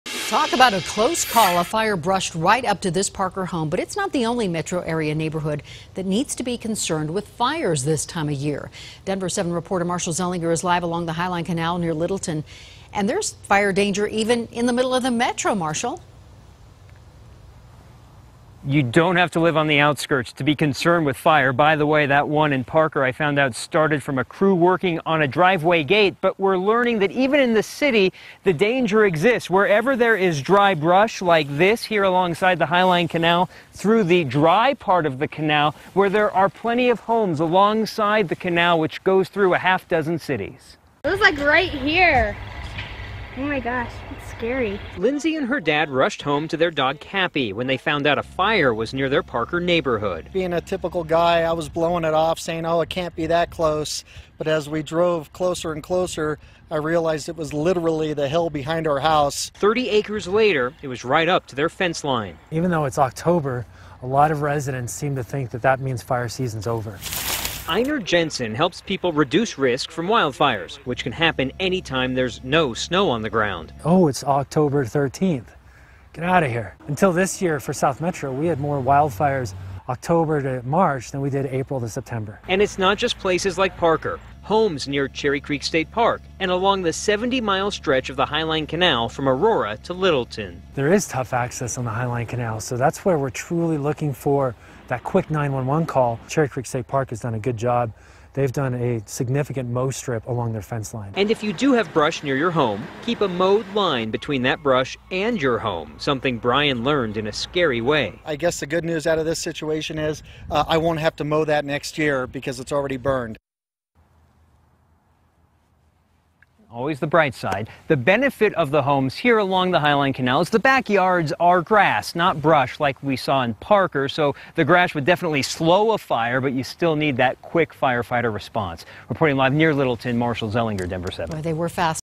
Brush fire comes close to homes in Parker
It was a close call for homeowners in Parker Thursday afternoon after a fast-moving brush fire came within feet of homes on Raintree Circle.